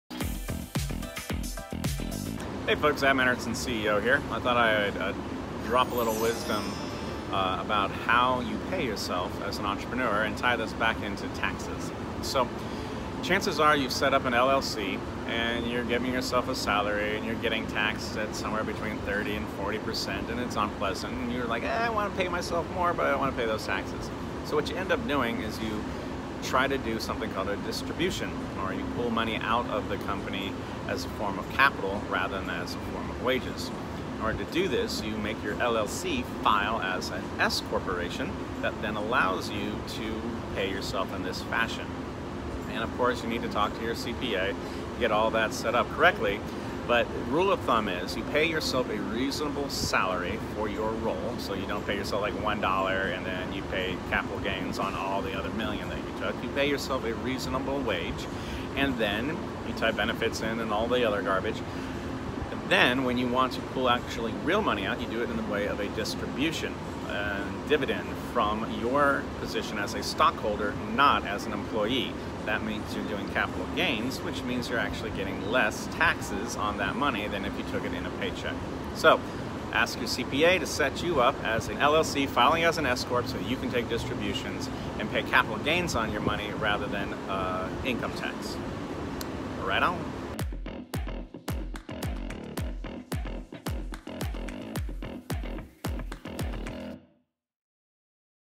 How Should Business Owners Pay Themselves? | Adam Anderson, CEO
Consult your CPA and/or attorney for firm advice on this topic...this video is just to get you thinking!

...but in general, (1) pay yourself a fair salary, covering taxes and benefits, etc.; and then (2) draw any additional cash out of the business via distributions, which are taxed less.

But HOW do you do #2? Well, talk to your CPA about filing your LLC as an S-Corporation (S-Corp). An S-Corp, under the right qualifying circumstances, will allow you to draw money out as capital, not as wages—which will incur lower taxes.

Right on.